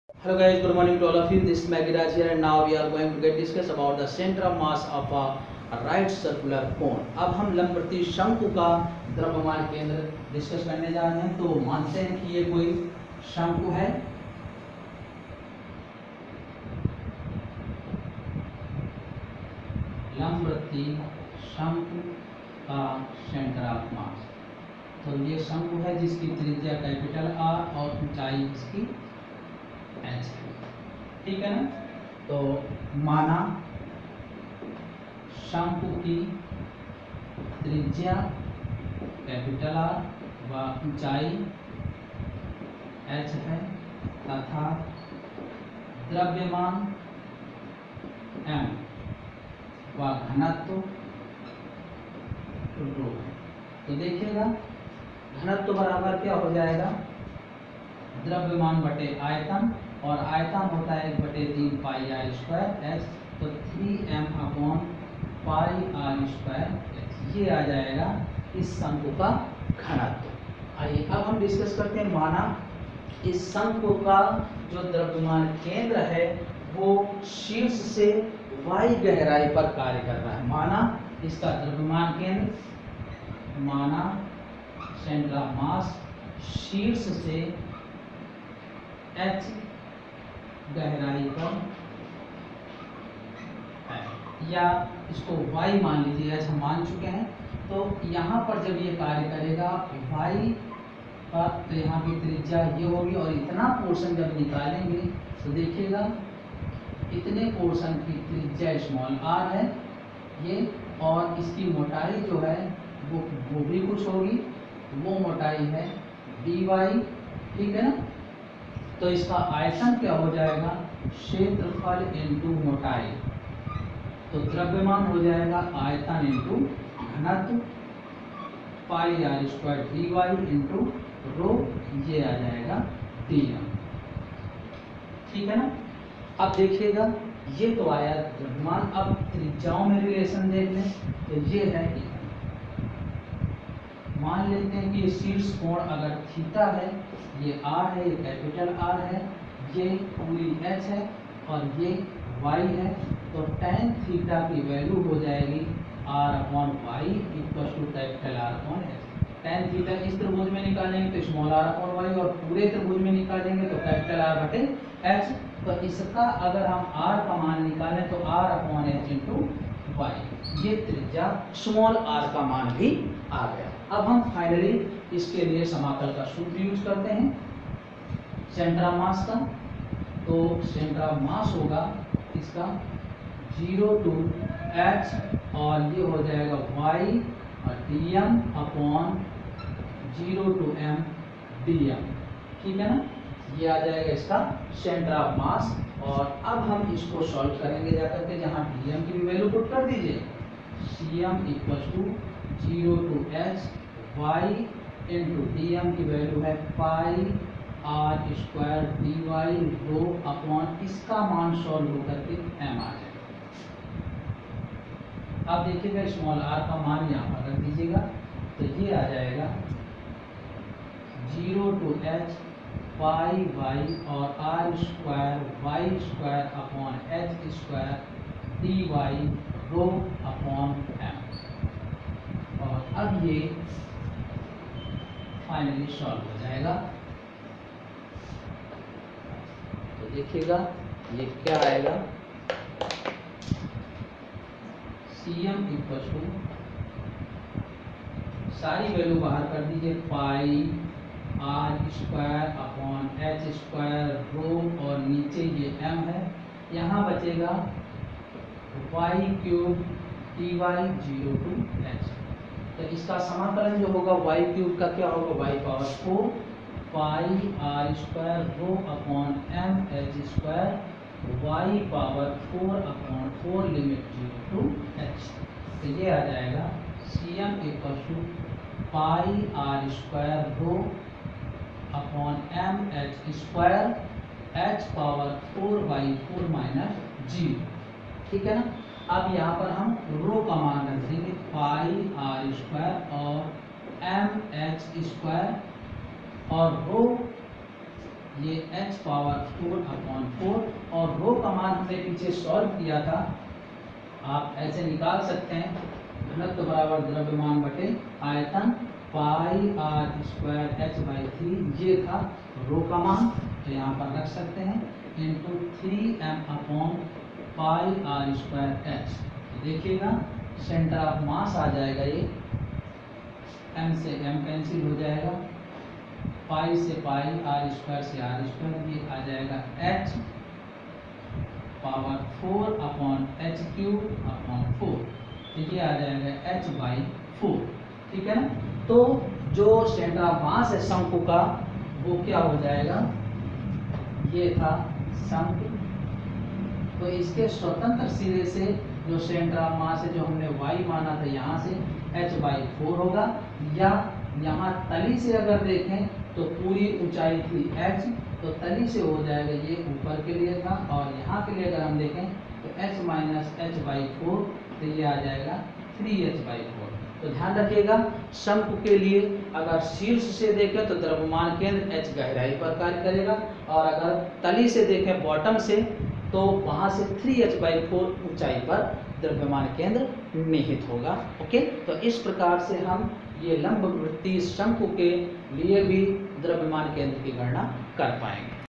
Physics by Maggi Sir (class XI)hindi medium
Topic..कणो का निकाय एवं घूर्णन गति (भाग 03)
शंकु का द्रव्यमान केन्द्र समाकलन विधि द्वारा ।